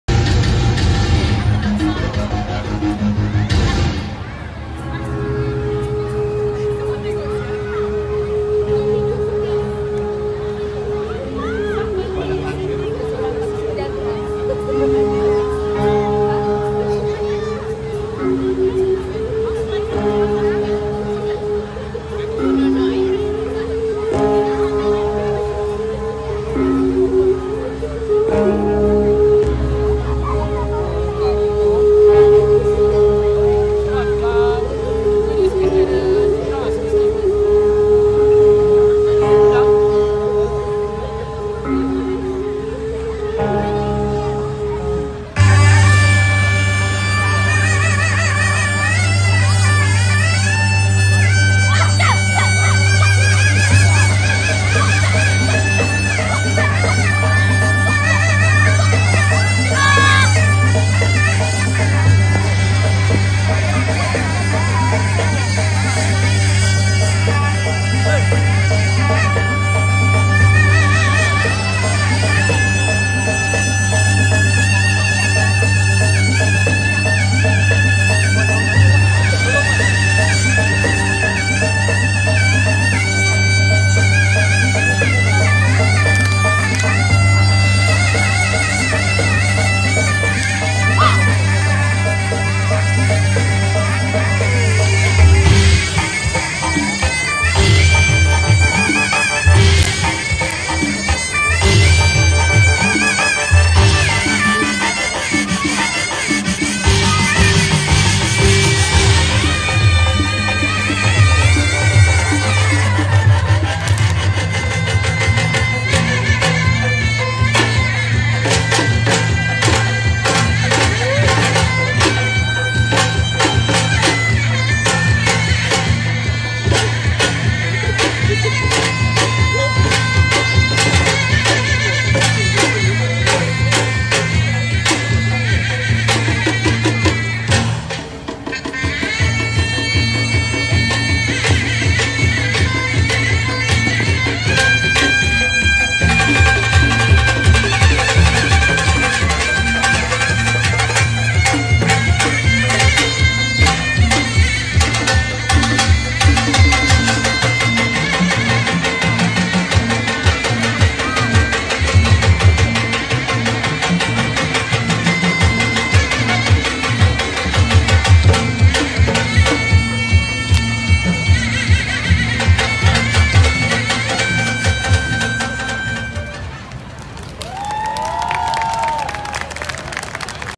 Flashmob by our national athletes at national day carnival 2018
On 11 August 2018 at cultural organized by tampines west hub comunnity center .